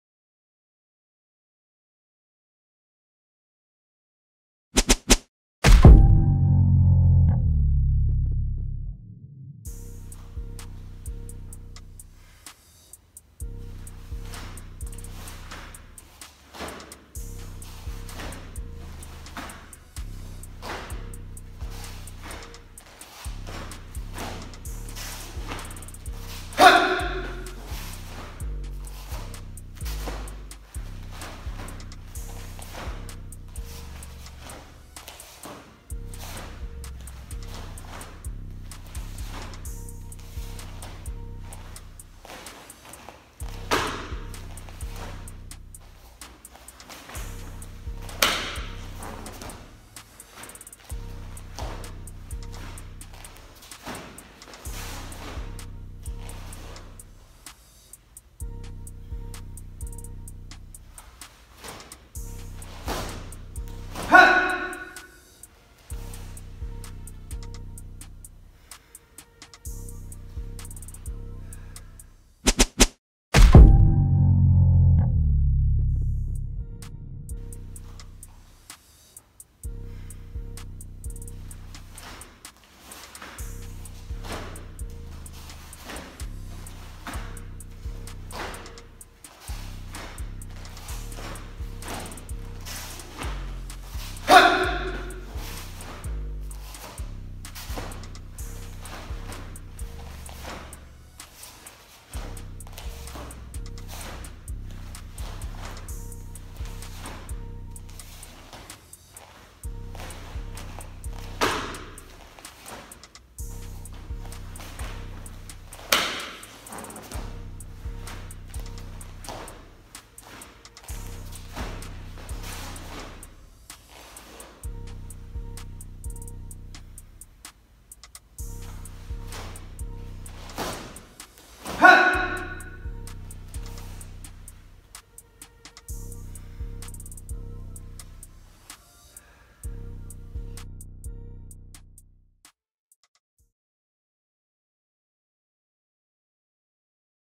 [PalGwe 8 Jang] Taekwondo Traditional poomsae.
The theme for the Palgwe foms centers on the
mutually opposing concepts in the universer
which are developing, growing, combining, and changing.
Furthermore, that the sky and earth, man and woman,
 and so on, all meet and depart according to
the rule of change and cooperation,
conflict in harmony, all of which create
and maintain balance in the universe.

 8 Jang means Kon in Palgwe,
which means the earth.
The earth is the basic ground on which
growing and breeding take place.